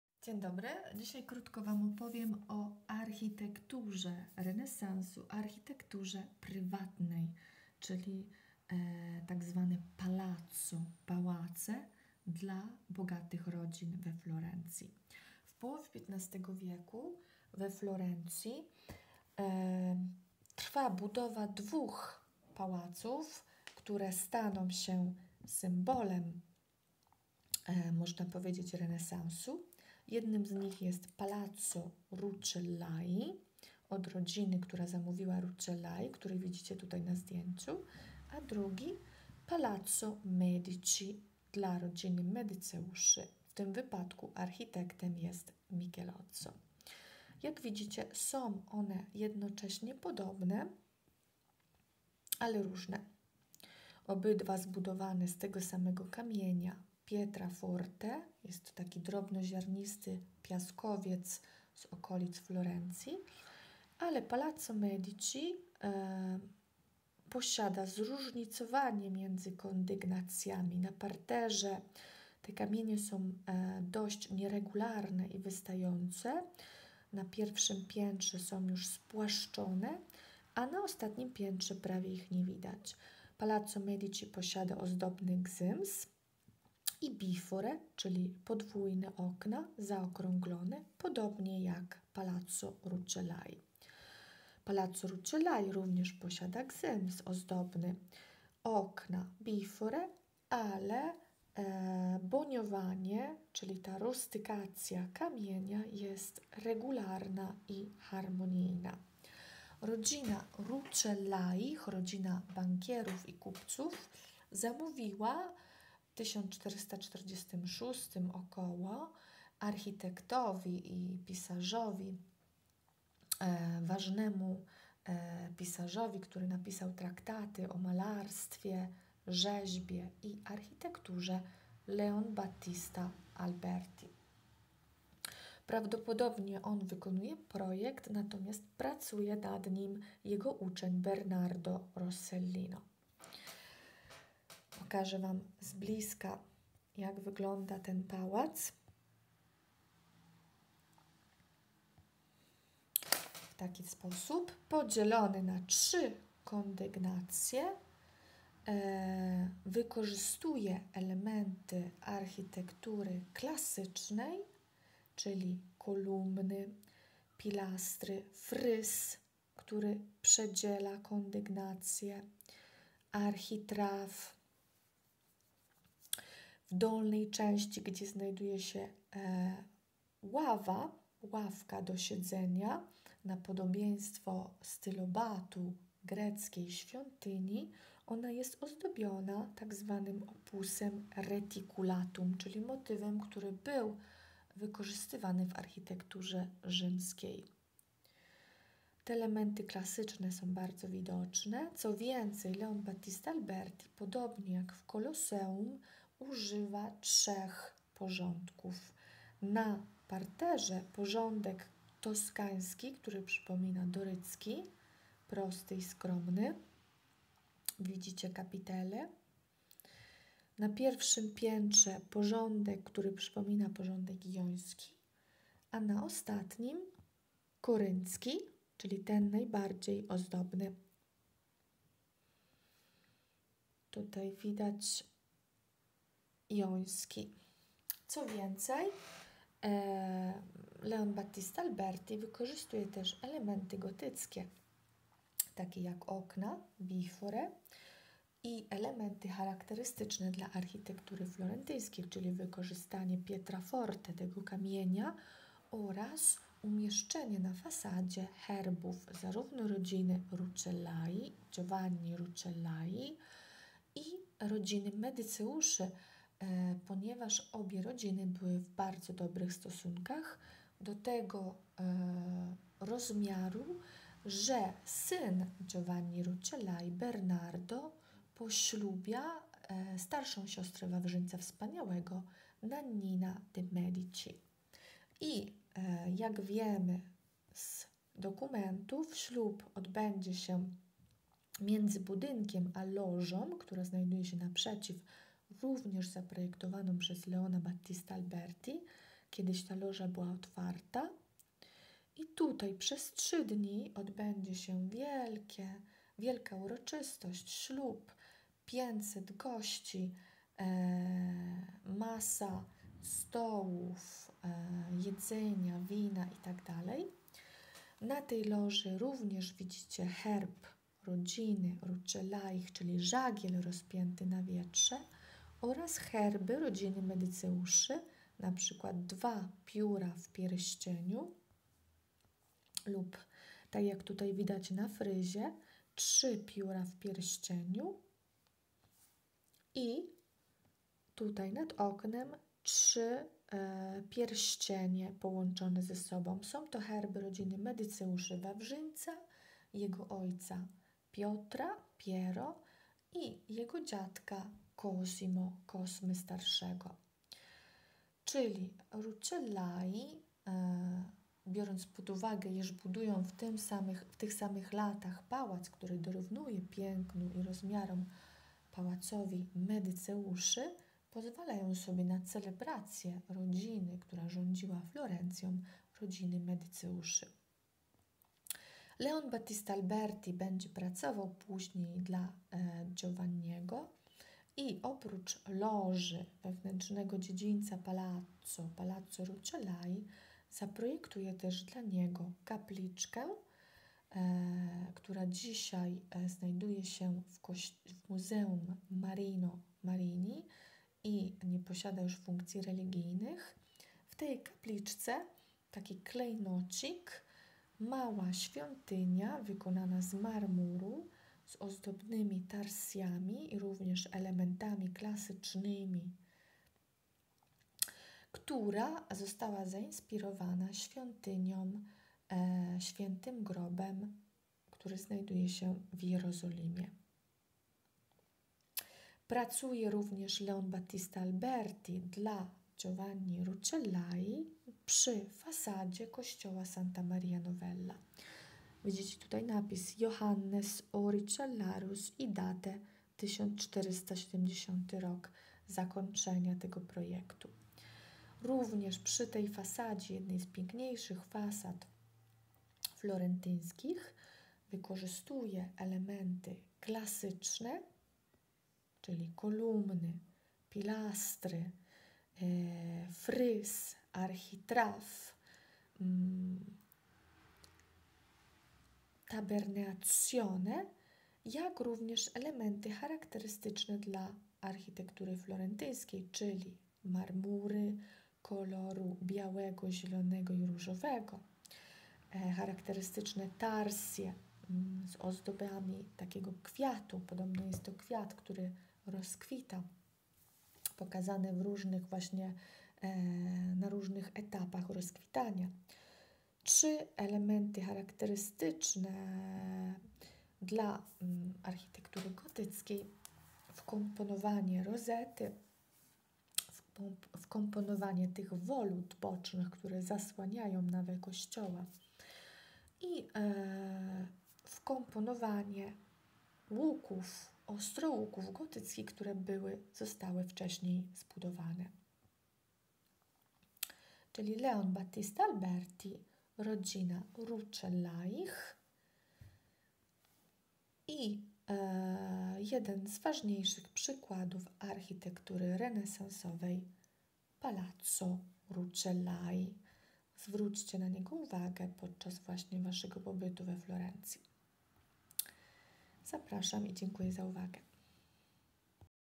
Spotkania ze sztuką, część 7
Palazzo Rucellai we Florencji. Leon Battista Alberti, Bernardo Rossellino. Architektura renesansu we Florencji.